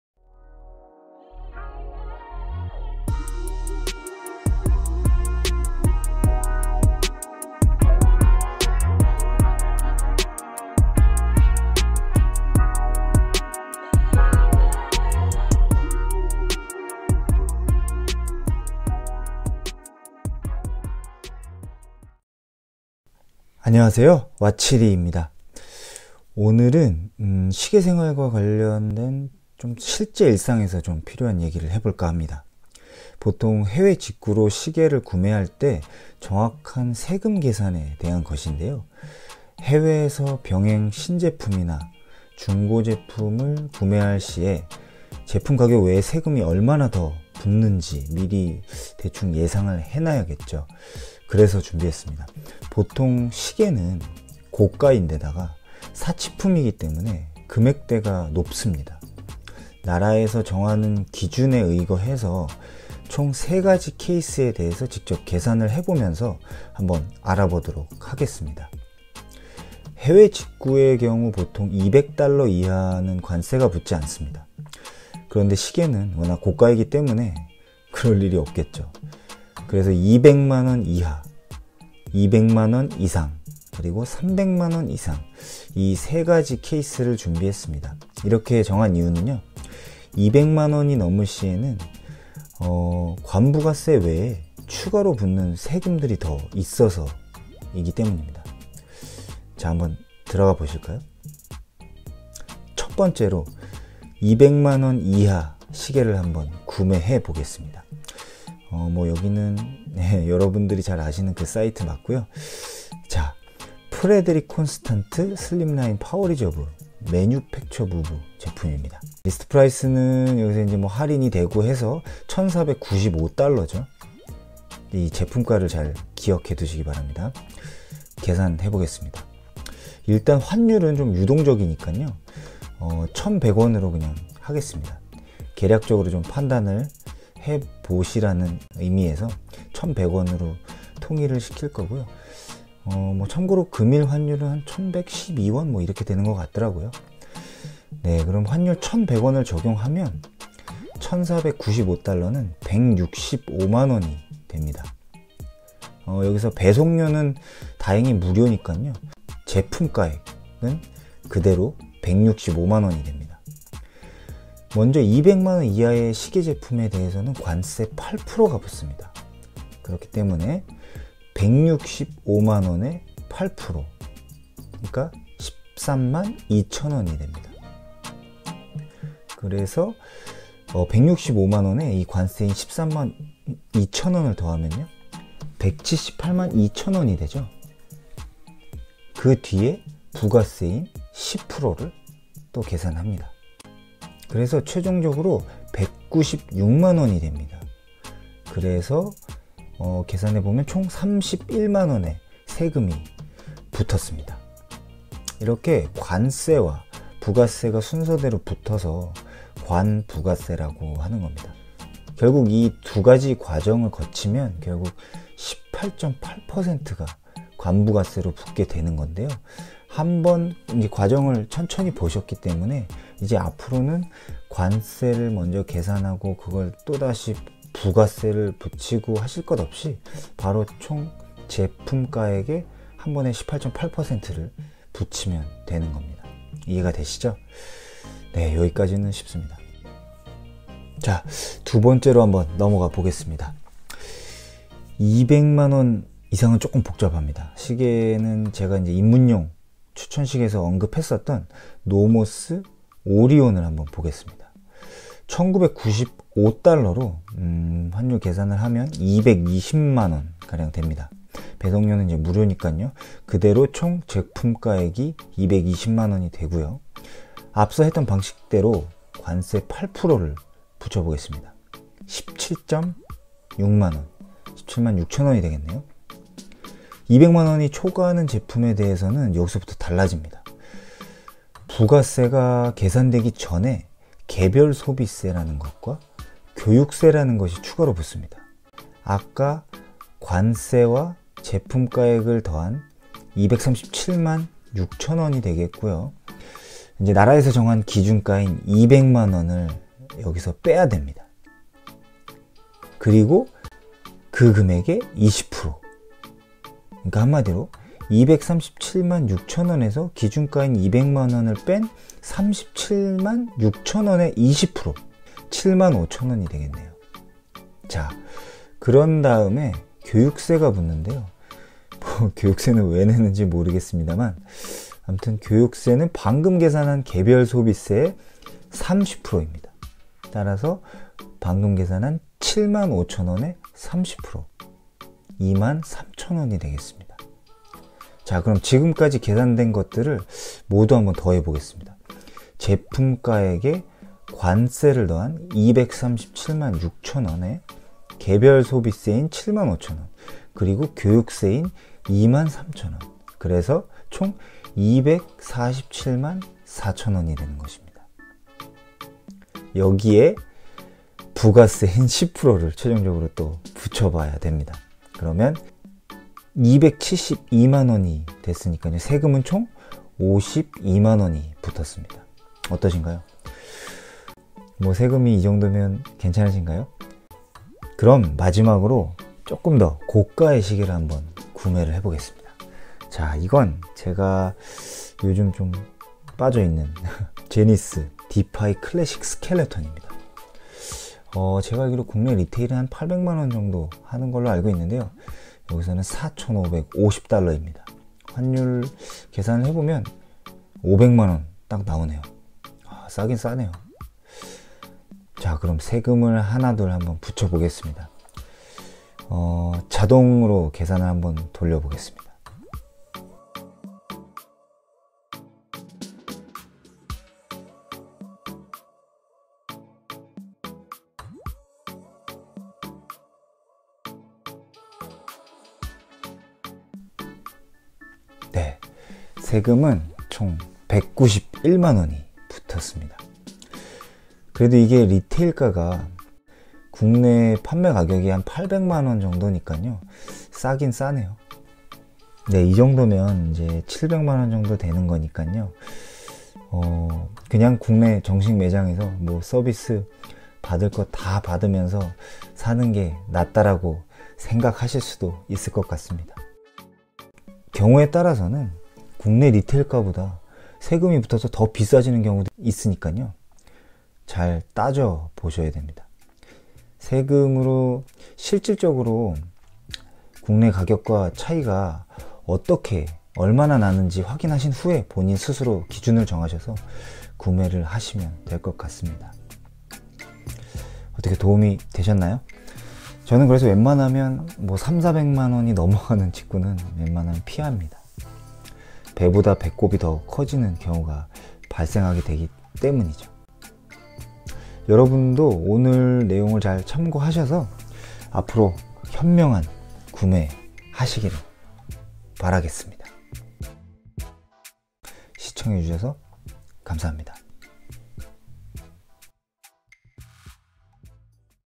시계 해외직구시 세금(관부가세) 계산법 - 200만원 이하/200만원 이상
* 볼륨 및 오디오 개선으로 재업합니다. 양해 부탁드립니다. 앞으로 추가 수정은 없습니다.

00:00 인트로
01:40 200만원 이하
04:00 200만원 이상
06:20 300만원 이상
07:35 아웃트로